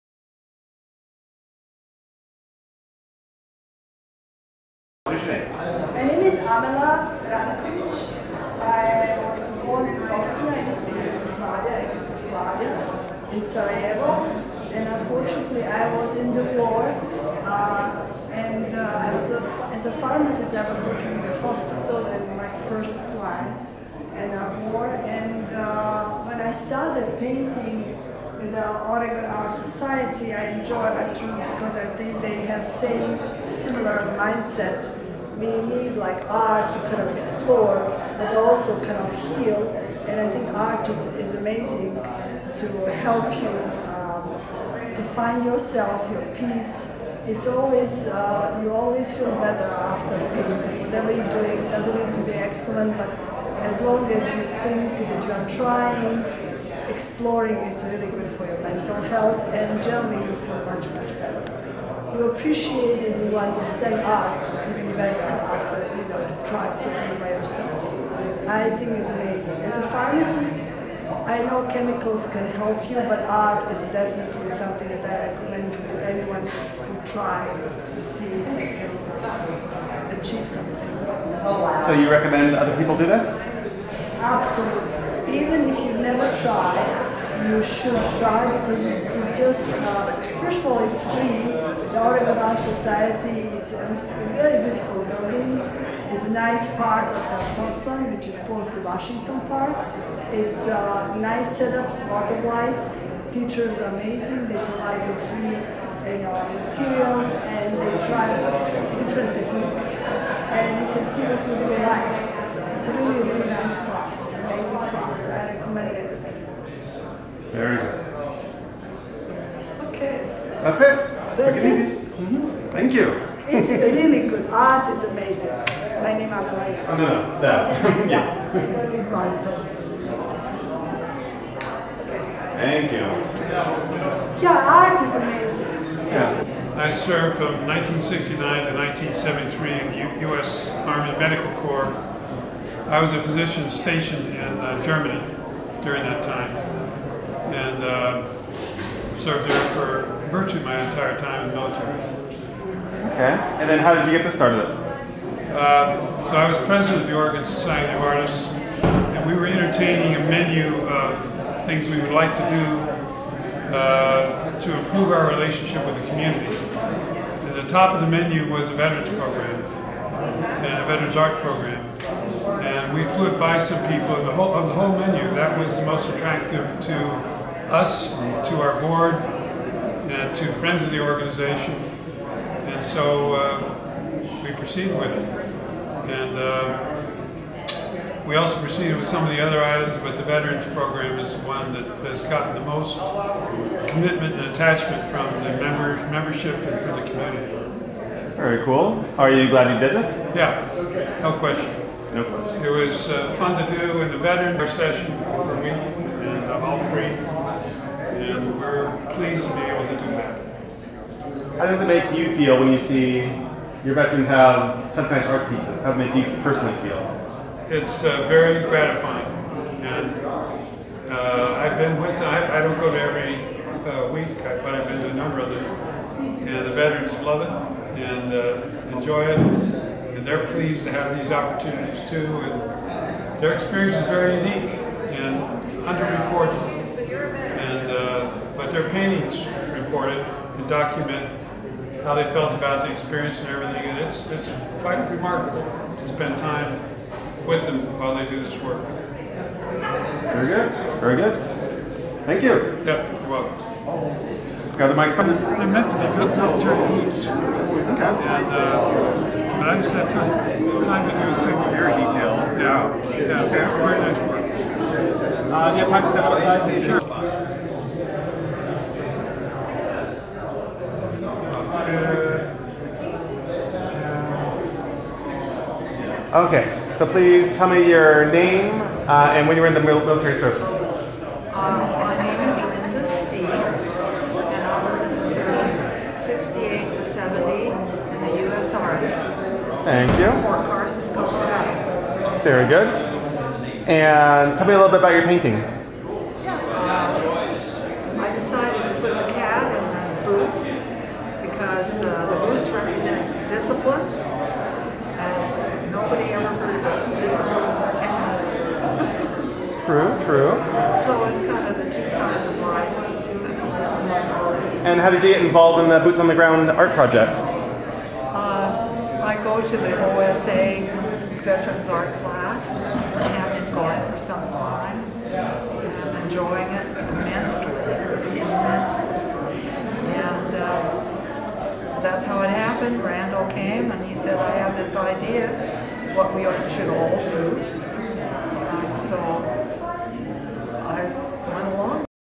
Boots on the Ground for Art interviews with Veterans
Boots on the Ground for Art interviews with Veterans at the VA hospital in Portland OR May 2024